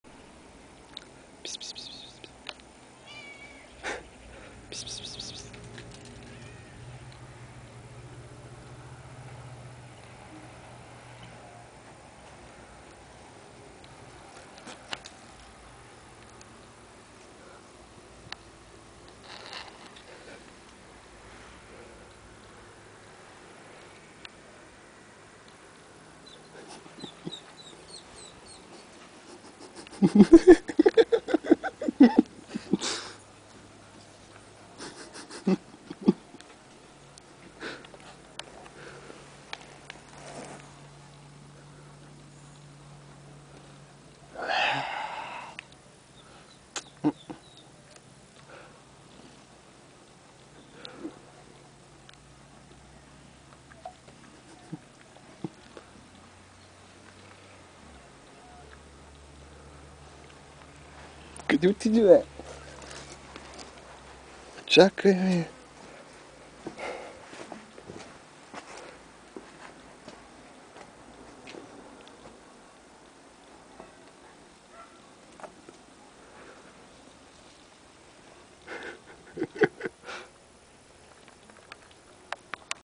lazy cat (Ancient)
a cat and a man, wondering what to do.